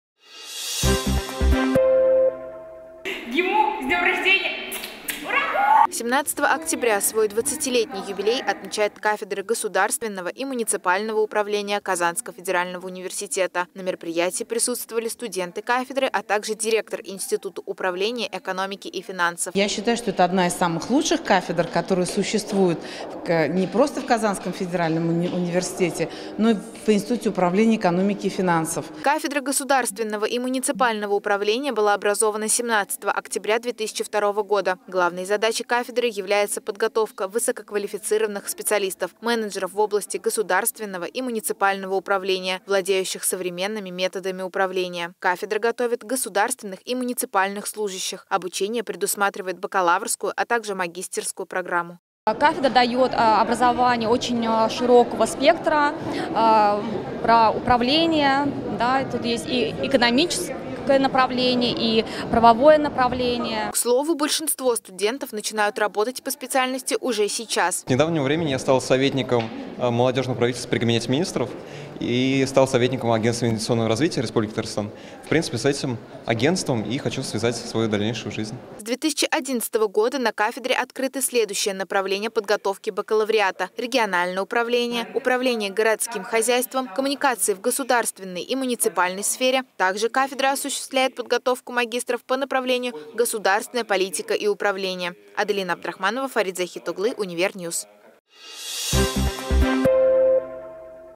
КАФЕДРЕ ГОСУДАРСТВЕННОГО И МУНИЦИПАЛЬНОГО УПРАВЛЕНИЯ 20 ЛЕТ
17 октября Кафедре государственного и муниципального управления Казанского федерального университета исполнилось 20 лет. Об истории кафедры, его работе и выпускниках - узнаете в сюжете.